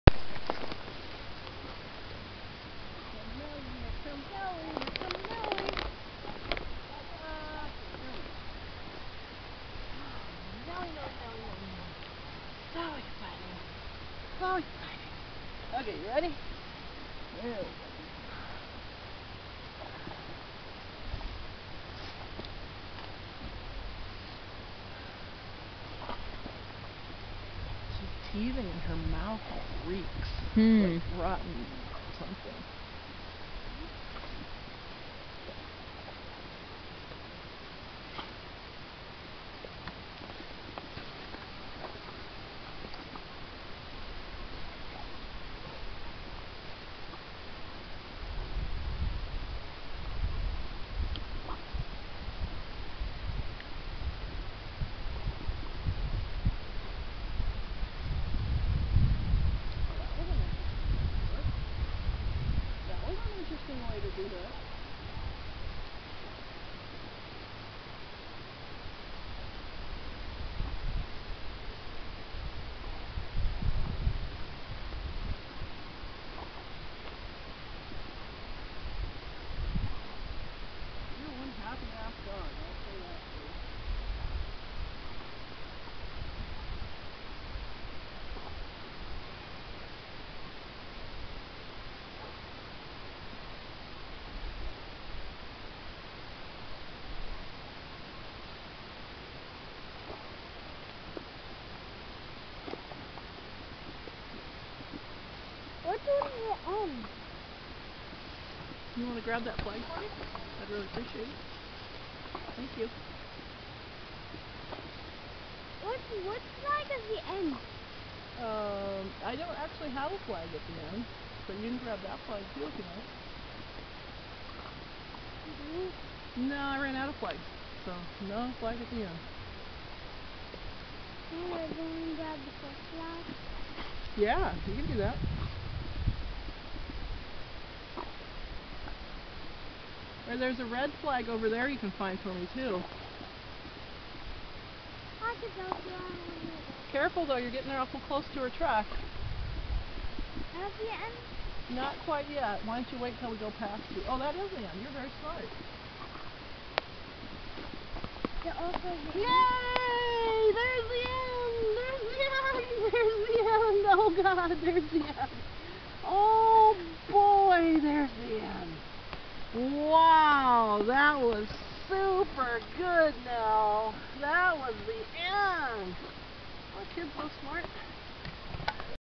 Boldog Mad Nell IPO tracking 5 months old pit bull
Here is Nell at 5 months, running a nice little track. I don't train "footstep", just "get down the track in a reasonably decent manner". : )  Nothing wrong with footstep tracking, it looks nice, I just don't want to spend that much time on one dog.  I also like to see a dog put a little "umph" into tracking, which the Germans don't like; they want mechanical... to each their own!